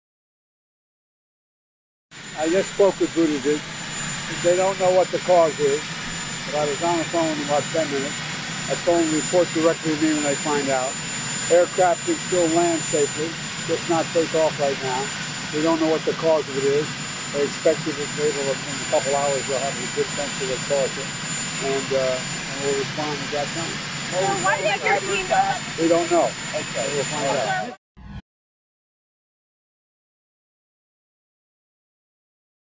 Breaking News | Biden orders FAA outage investigation that grounded domestic flights | Falcon Media
Biden orders FAA outage investigation that grounded domestic flights

Falcon Media News